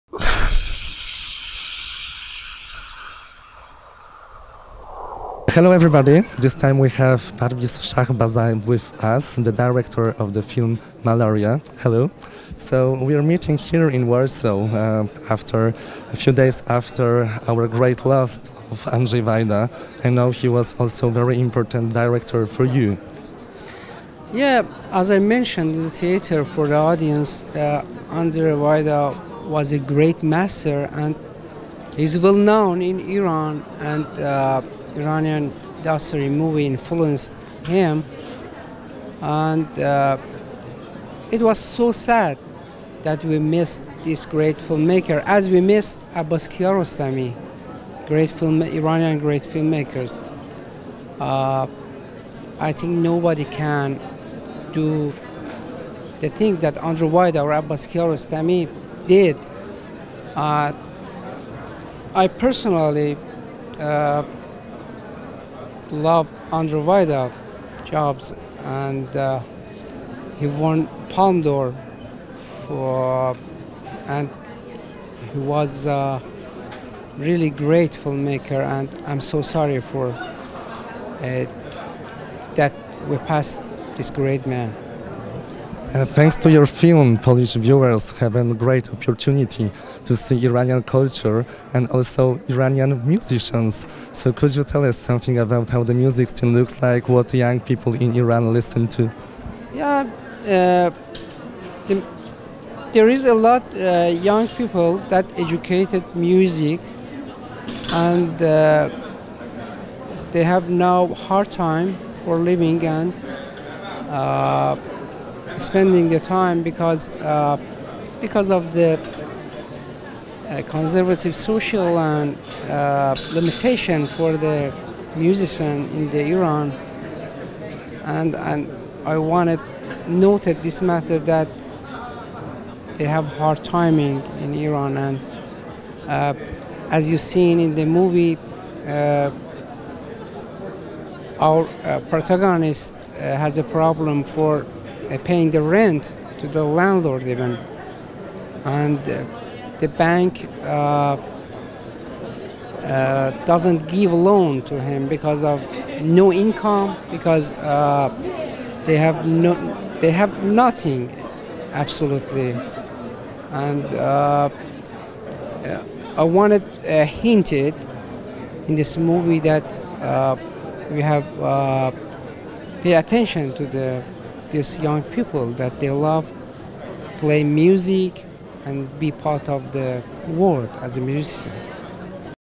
WFF 2016: Parviz Shahbazi, reżyser MALARIA
Reżyser: Parviz Shahbazi
Irański reżyser, scenarzysta i montażysta. Urodził się w 1962 roku w Teheranie. Absolwent szkoły filmowej w Teheranie. Autor opowiadań, montażysta i reżyser wielu filmów krótkometrażowych. Jego debiut fabularny "Traveller from the South" (1997), a także drugi film "Whispers" (2000) zostały pokazane na licznych festiwalach, gdzie zyskał międzynarodowe uznanie i zdobył kilka nagród. W 2003 roku jego trzeci film "Deep Breath" został pokazany w sekcji Directors' Fortnight na festiwalu w Cannes oraz na festiwalu w Pusan, gdzie otrzymał nagrodę FIPRESCI. "Malaria" jest jego najnowszym filmem fabularnym.

Iranian director, screenwriter and editor. Born in 1962 in Tehran, he is a filmmaking graduate of the Tehran Cinema College. Short story writer, film editor and director of many short films. His first feature film, “Traveller from the South” (1997), as well as his second film “Whispers” (2000) were screened at countless festivals, bringing him international recognition and several awards along the way. In 2003 his third film, “Deep Breath”, was selected for the Directors' Fortnight section of the Cannes festival and was screened in Pusan, wining the FIPRESCI award. “Malaria” is his latest feature.

Opis filmu:

Młoda dziewczyna wysyła informację do swojego ojca, że została uprowadzona. Prosi go, żeby przekazał pieniądze na okup. Spanikowany ojciec wraz z jej braćmi jadą do Teheranu, by odszukać dziewczynę. Nie są jednak świadomi tego, że tak naprawdę uciekła ona ze swoim chłopakiem, aby dobrze się bawić, a teraz spędza czas w towarzystwie przypadkowo spotkanych muzyków ulicznych. Kiedy lider grupy zostaje aresztowany, zakochana dziewczyna wkracza na niepewną ścieżkę.

A young girl informs her father she’s been kidnapped and asks him to bring the ransom money. Panicked, her father and brothers go to Tehran to look for her. They are unaware, however, that the girl has in fact eloped with her boyfriend to have fun, and now spends her time in the company of a band of street musicians she met by accident. Following the arrest of the band leader and full of love, the young girl takes an uncertain path.